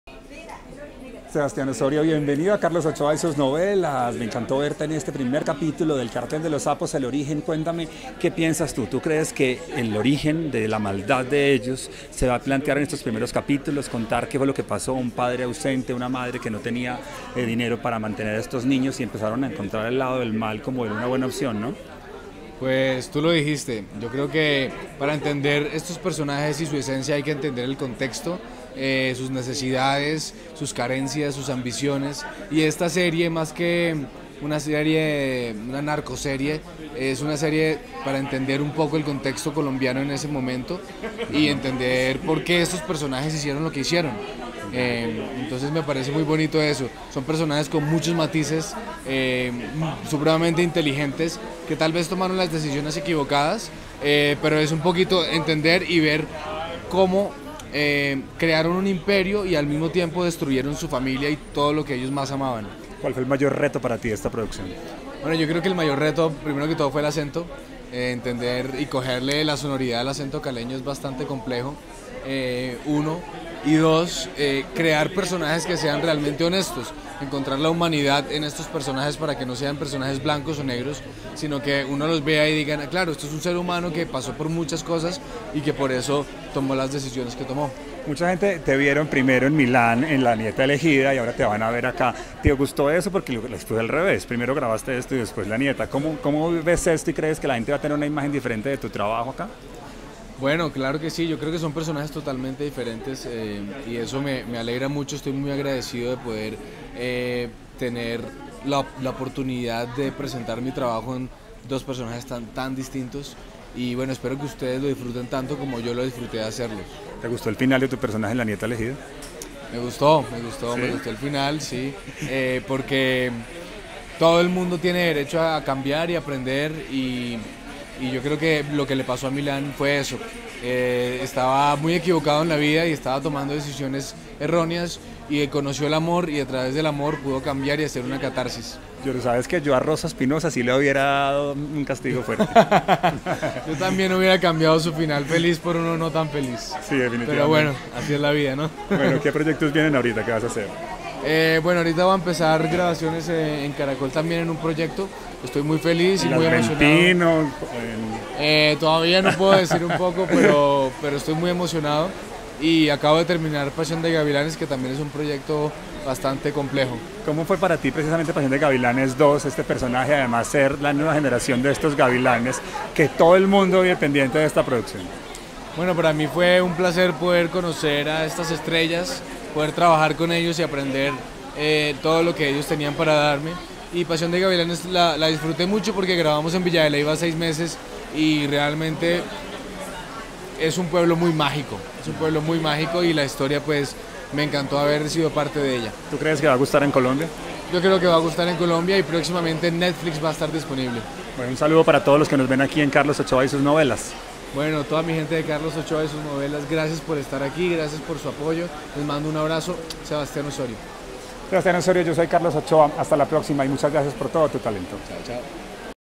SEBASTIAN OSORIO DE EL CARTEL DE LOS SAPOS EL ORIGEN A PASIÓN DE GAVILANES 2
El actor Sebastian Osorio habla de sus personajes en El Cartel de los Sapos el Origen, Pasión de Gavilanes 2 y la nieta elegida.